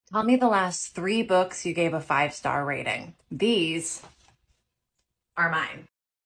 The Last 3 Nonfiction Books I gave 5 Stars To
These are the last three books that I gave great ratings for!
Hannibal by Philip Freeman
Birding Basics by Noah Strycker
Bird Way by Jennifer Ackerman

Lots of science, nature, and history among other topics. Plenty of long form content as well!

Etsy Shop